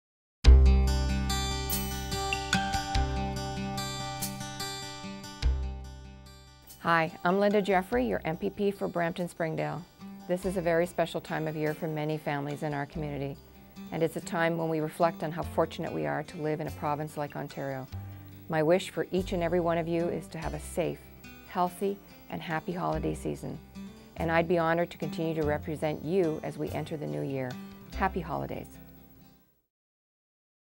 Linda Jeffrey Holiday Greetings 2012
A holiday greeting from Hon. Linda Jeffrey, Member of Provincial Parliament for Brampton-Springdale.